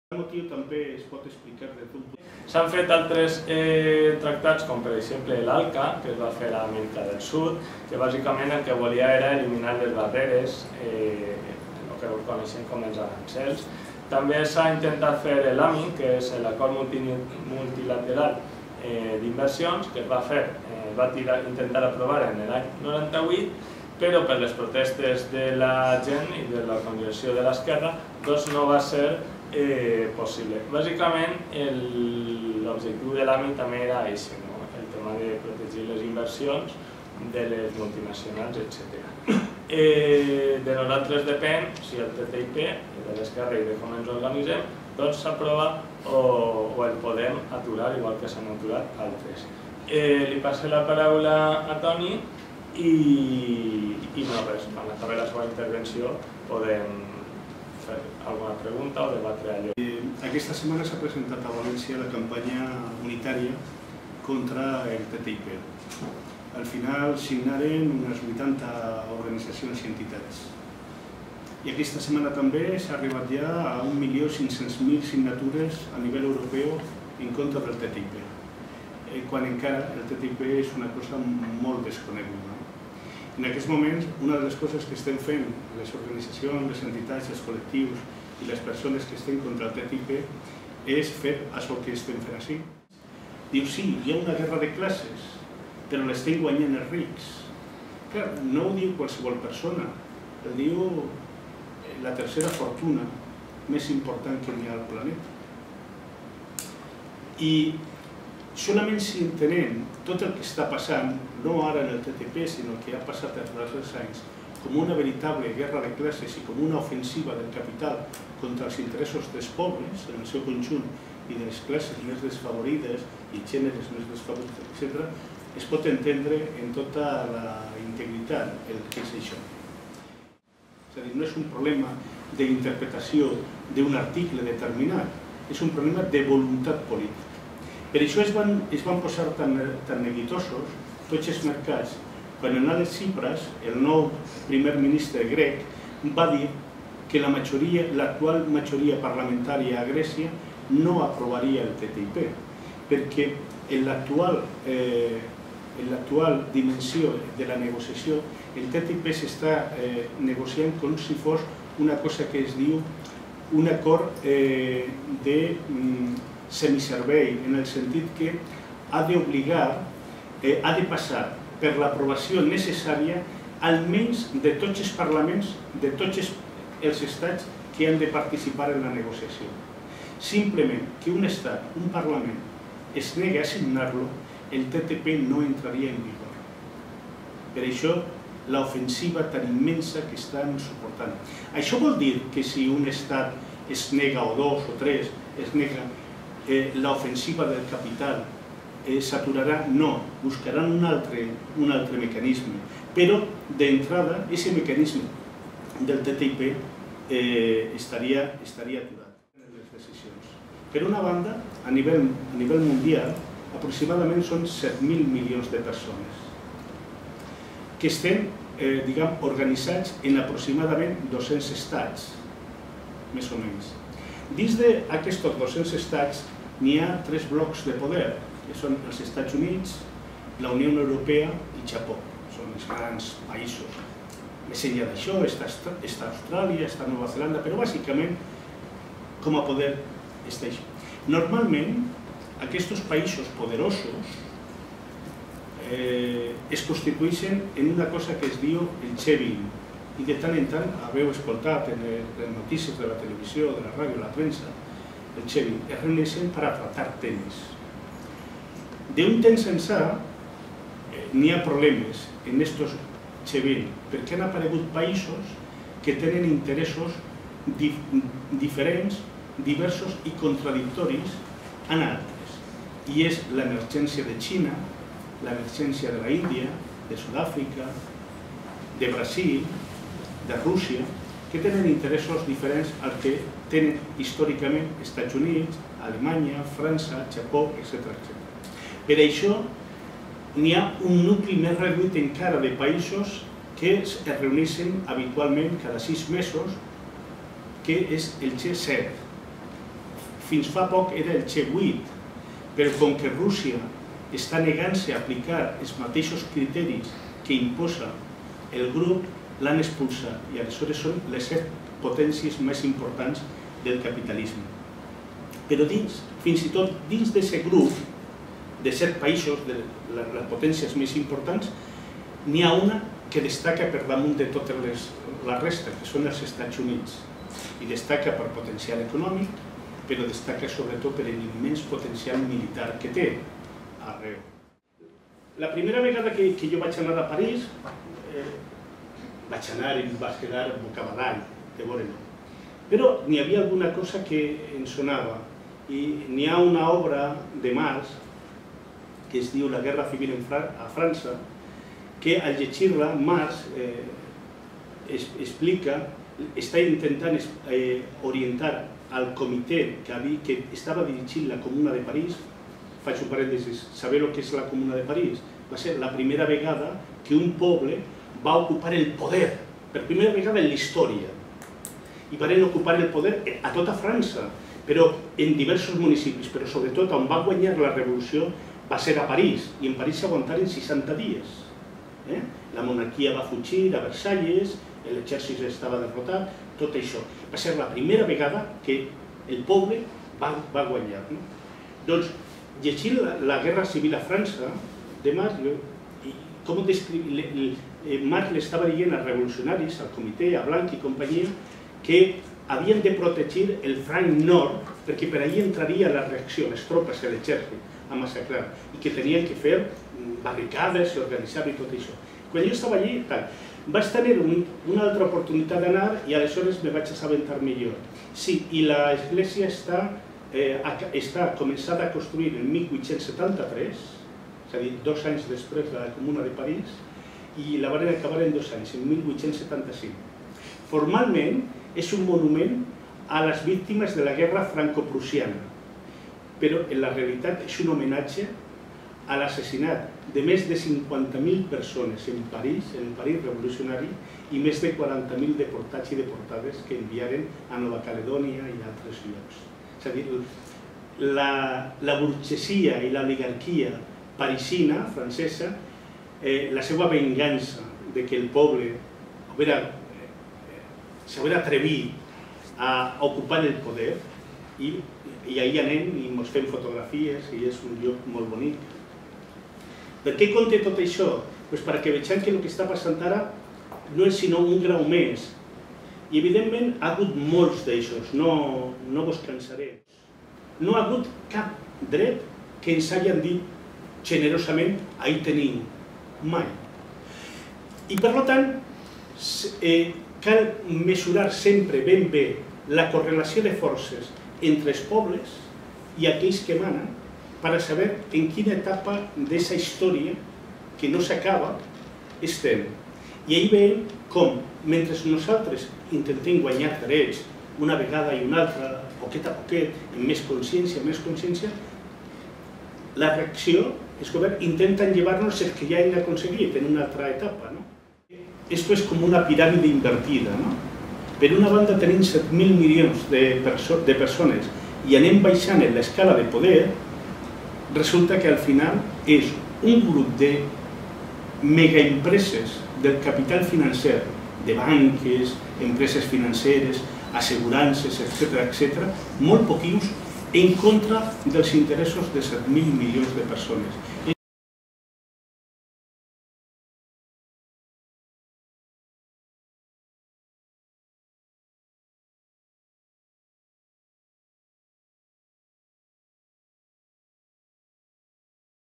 Com pot afectar als nostres drets el Tractat de Comerç i Inversions (TTIP) entre Europa i els EUA?
a càrrec de
Toni Infante (CUP Almàssera)
Eduard Garcia (CUP Biar)

Biar, 27 de gener de 2015